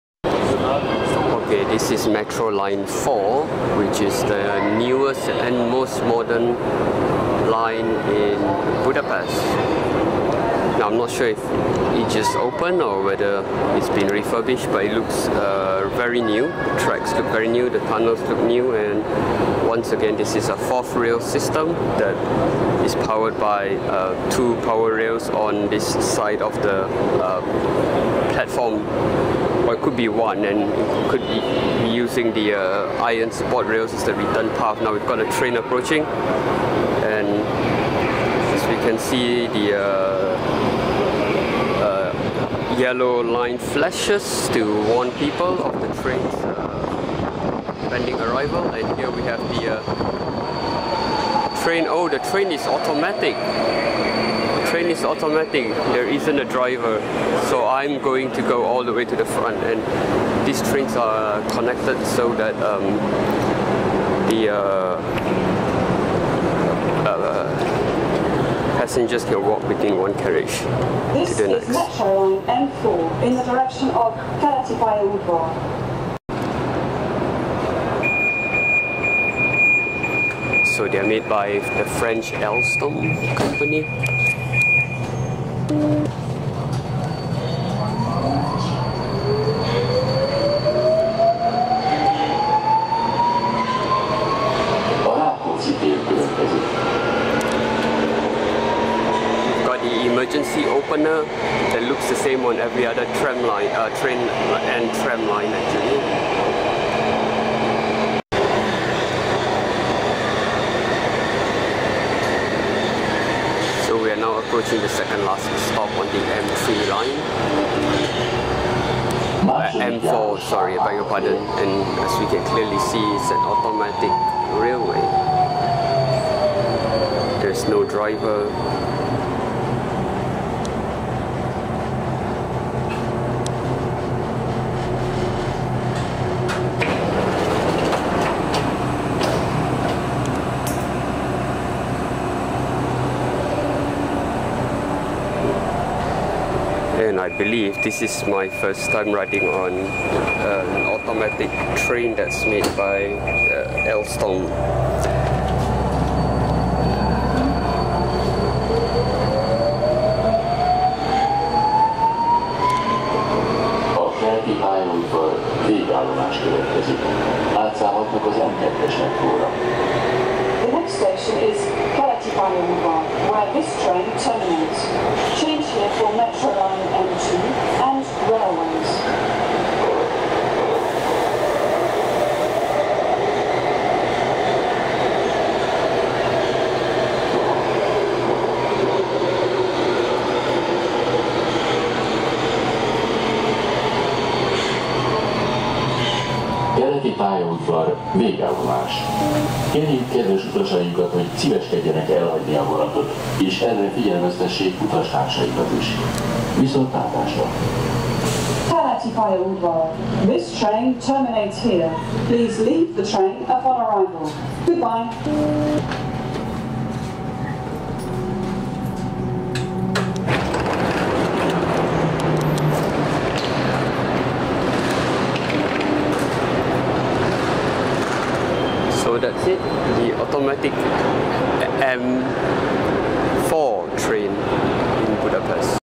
Budapest's newest and most advanced Metro Line 4 (green)
Metro Line 4 is the newest and most advanced line on the network. This is the only line in Budapest that run automatic (driver-less) trains.

The stations have sliding glass doors at the platforms and they are very bright, large and spacious.

These trains run on 750 Volts DC 3rd rail. They are of course fully enclosed, air conditioned and have very modern and clean interiors.

This line started serving Bud when it opened on 28th March in 2014.